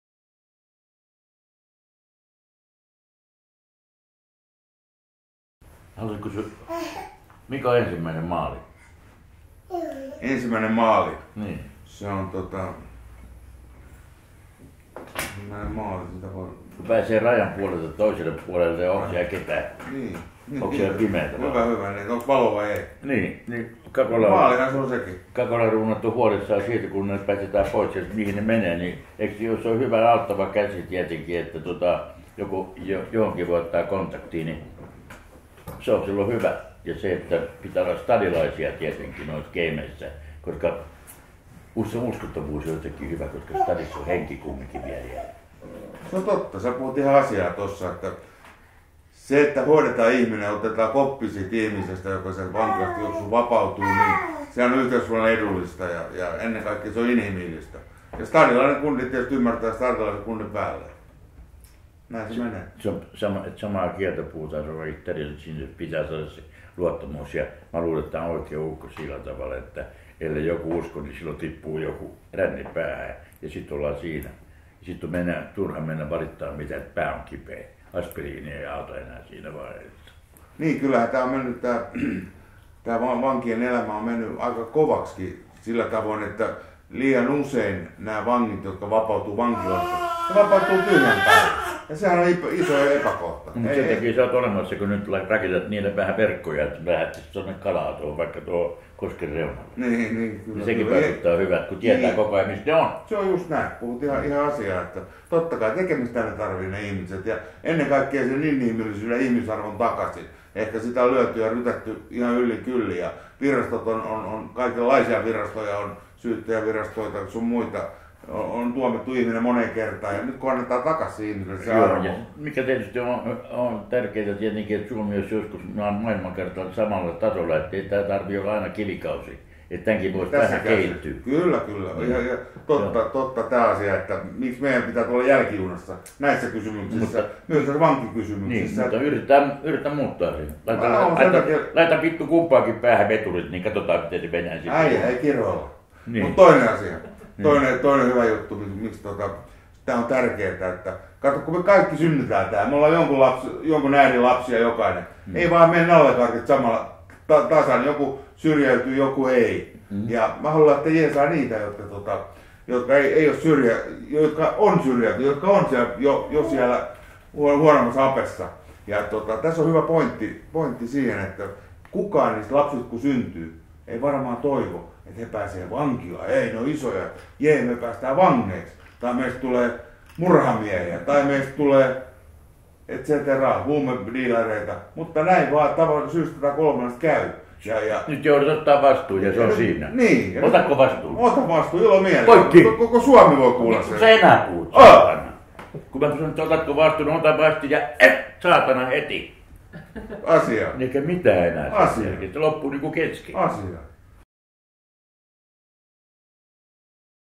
Jukan VLog 4. Jukka ja Remu
Jukka ja Remu Aaltonen keskustelevat Jukan vaaliteemoista värikkäästi.

Videokuvaus ja editointi: Juha Seila